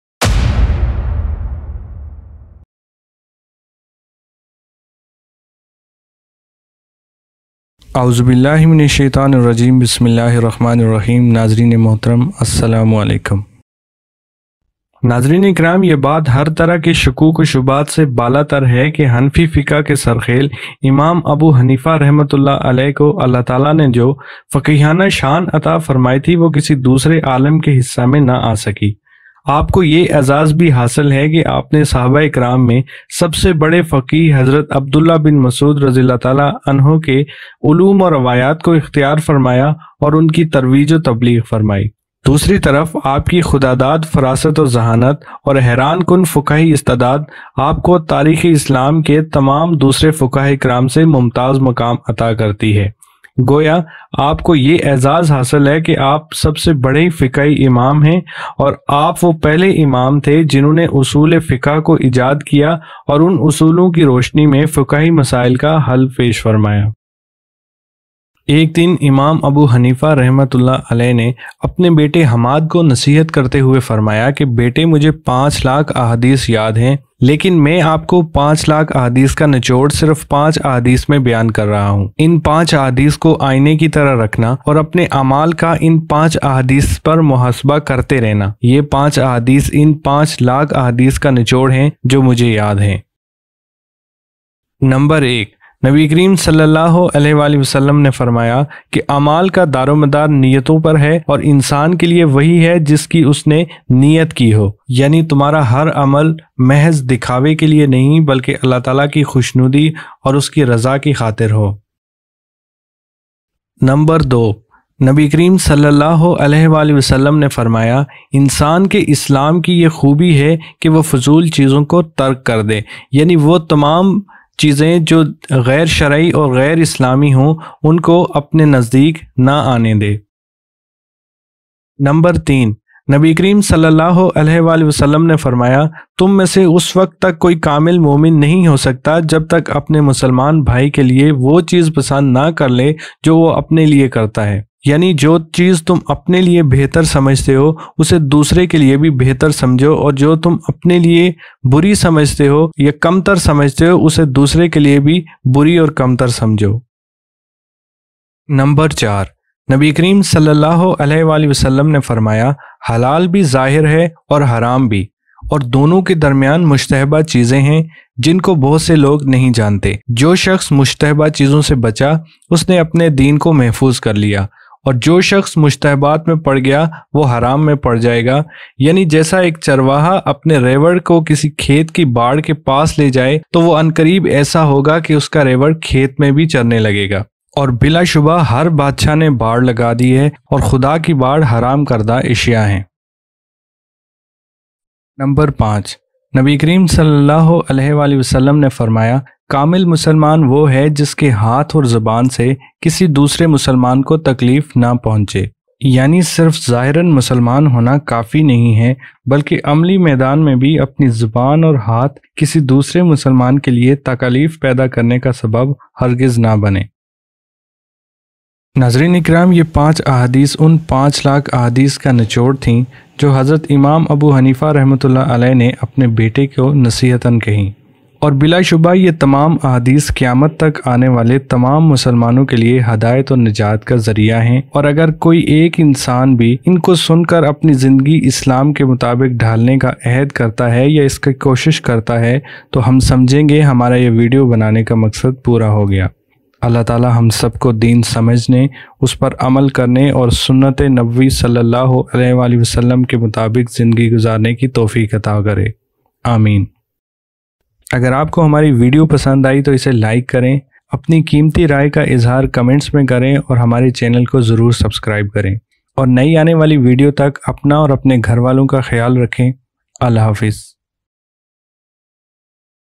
Imam Abu Hanifa Ki Apne Betay Ko Nasihat | 5000 Hadiths Ka Nachor | Urdu / Hindi
Hi Everyone! Asalam-u-Alaikum

In this video we will tell the advices of Imam Abu Hanifa (RA) to His Son . We hope you will like this video, if it does, press LIKE button and do SHARE.
Special thanks to pixabay

Thanks.

Thanks